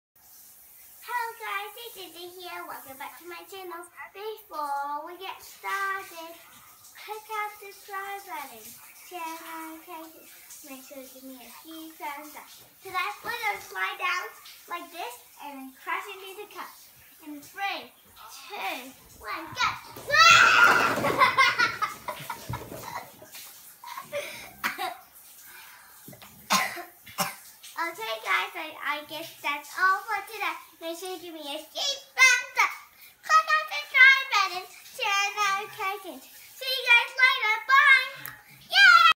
Paper cup tower
Another paper cup tower before going to bed